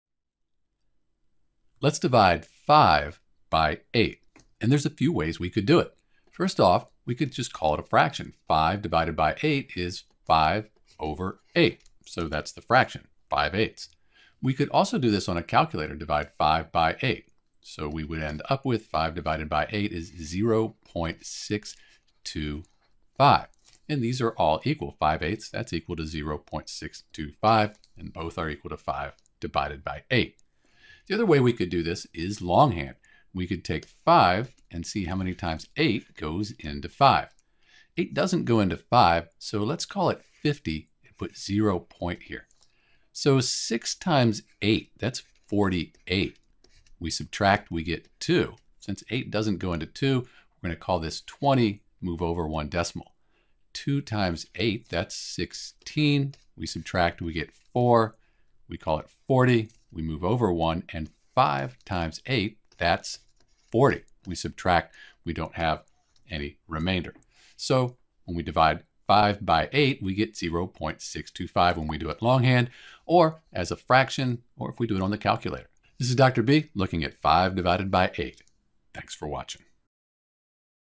5 divided by 8 (5 ÷ 8)
The general process for division:
1. make an educated guess
2. multiplication
3. subtraction
4. bringing down the next digit (when necessary)

Note that division is the inverse operation of multiplication.  So we can check our work for 5 divided by 8 by multiplying the answer (0.625) times eight.   If this equals five we did the division correctly.